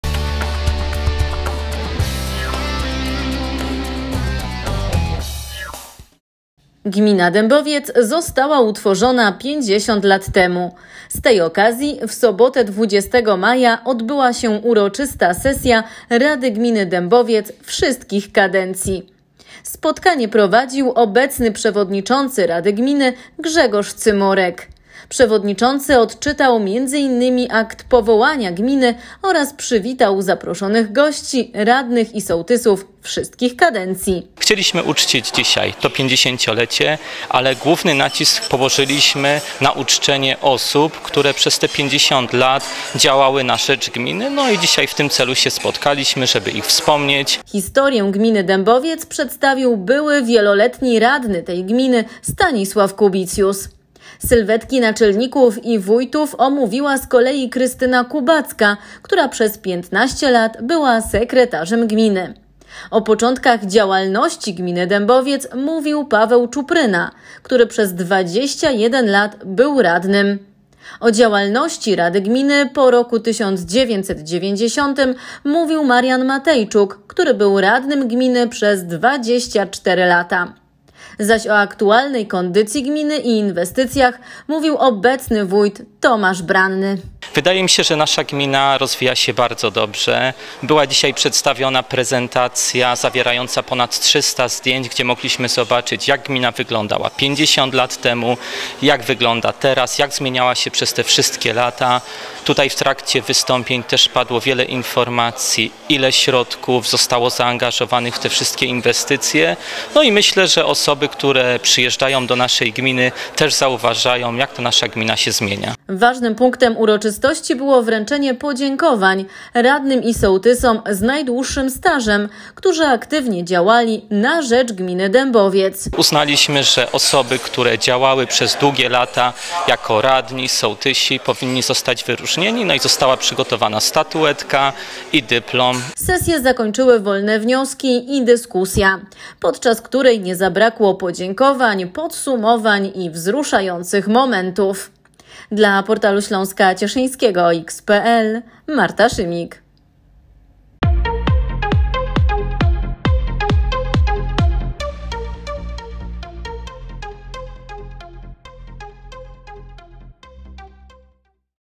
50 lat Gminy Dębowiec
Rok 2023 jest nie tylko rokiem najstarszego osadnictwa na Śląsku Cieszyńskim potwierdzonego w źródłach historycznych, ale również rokiem jubileuszowym dla wielu gmin. 50 lat temu została utworzona m.in. Gmina Dębowiec. Z tej okazji 20 maja w sali OSP w Dębowcu odbyła się uroczysta Sesja Rady Gminy Dębowiec wszystkich kadencji.